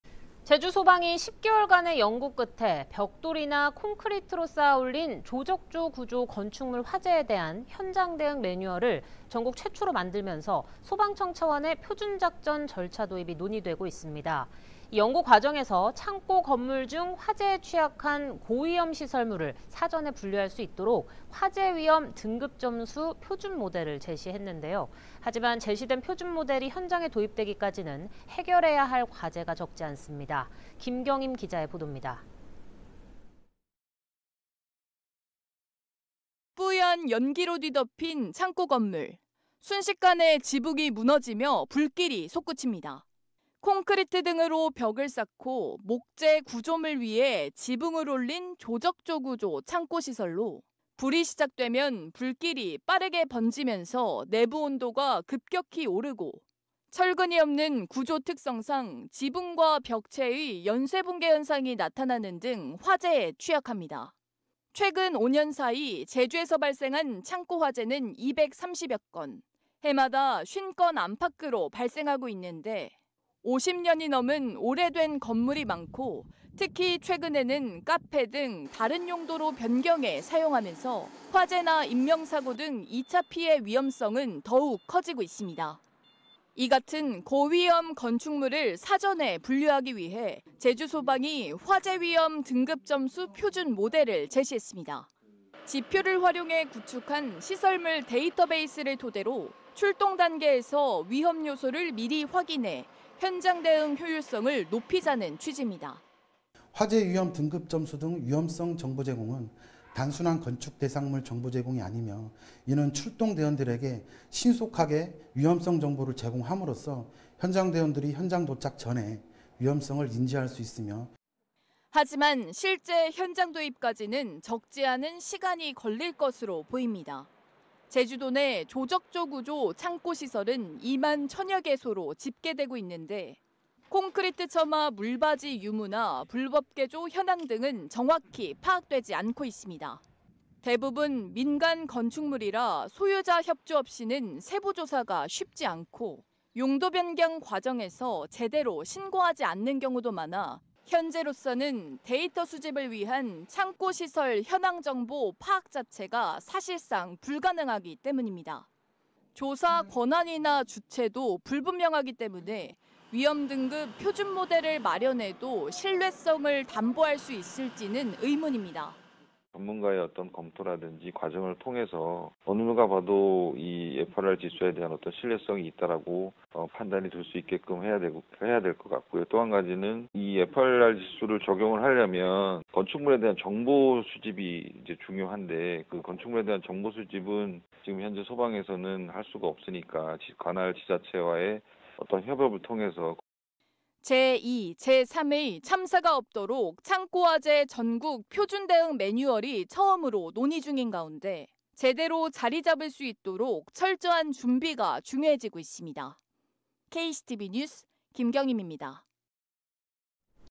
'화재 고위험 건축물 사전 분류'…과제도 많아
제주 소방이 10개 월 간의 연구 끝에 벽돌이나 콘크리트로 쌓아 올린 조적조 구조 건축물 화재에 대한 현장 대응 매뉴얼을 전국 최초로 만들면서 소방청 차원의 표준작전 절차 도입이 논의되고 있습니다.

연구 과정에서 조적조 창고 건물 중 화재에 취약한 고위험 시설물을 사전에 분류할 수 있도록 화재위험 등급 점수 표준 모델을 제시했는데요.

하지만 제시된 표준 모델이 현장에 도입되기까지는 해결해야 할 과제가 적지 않습니다.

뿌연 연기로 뒤덮인 창고 건물.

순식간에 지붕이 무너지며 불길이 솟구칩니다.

콘크리트 등으로 벽을 쌓고 목재 구조물 위에 지붕을 올린 조적조 구조 창고 시설로,

불이 시작되면 불길이 빠르게 번지면서 내부 온도가 급격히 오르고,

철근이 없는 구조 특성상 지붕과 벽체의 연쇄 붕괴 현상이 나타나는 등 화재에 취약합니다.

최근 5년 사이 제주에서 발생한 창고화재는 230여 건.

해마다 50건 안팎으로 발생하고 있는데, 50년이 넘은 오래된 건물이 많고 특히 최근에는 카페 등 다른 용도로 변경해 사용하면서 화재나 인명 사고 등 2차 피해 위험성은 더욱 커지고 있습니다.

이같은 고위험 건축물을 사전에 분류하기 위해 제주 소방이 화재 위험 등급 점수 표준 모델을 제시했습니다.

지표를 활용해 구축한 시설물 데이터 베이스를 토대로 출동 단계에서 위험 요소를 미리 확인해 현장 대응 효율성을 높이자는 취지입니다.

[정철 / 제주동부소방서 소방행정팀 소방위]
"화재 위험 등급 점수 등 위험성 정보제공은 단순한 건축물 대상물 정보 제공이 아니며, 이는 출동 대원들에게 신속하게 위험성 정보를 제공함으로써 현장대원들이 현장 도착 전에 위험성을 인지할 수 있으며."

하지만 실제 현장 도입까지는 적지 않은 시간이 걸릴 것으로 보입니다.

제주도내 조적조 구조 창고시설은 2만 1천여 개소로 집계되고 있는데, 콘크리트 처마 물받이 유무나 불법 개조 현황 등은 정확히 파악되지 않고 있습니다.

대부분 민간 건축물이라 소유자 협조 없이는 세부 조사가 쉽지 않고, 용도 변경 과정에서 제대로 신고하지 않는 경우도 많아 현재로서는 데이터 수집을 위한
창고 시설 현황 정보 파악 자체가 사실상 불가능하기 때문입니다.

조사 권한이나 주체도 불분명하기 때문에 위험 등급 표준 모델을 마련해도 신뢰성을 담보할 수 있을지는 의문입니다.

[송영호 / 대전과학기술대학교 소방산업안전관리학과 교수]
"전문가의 검토 과정 등을 통해서 어느 누가 봐도 이 FRR(화재위험등급) 지수에 대한 어떤 신뢰성이 있다고 판단될 수 있게끔 해야 될 것 같고요. 또 한 가지는 일단 이 FRR 지수를 적용을 하려면 건축물에 대한 정보 수집이 중요한데 건축물에 대한 정보 수집은 지금 현재 소방에서는 할 수가 없으니까 관할 지자체와의 어떤 협업을 통해서."

제2, 제3의 참사가 없도록 창고 화재 전국 표준 대응 매뉴얼이 처음으로 논의 중인 가운데 제대로 자리잡을 수 있도록 철저한 준비가 중요해지고 있습니다.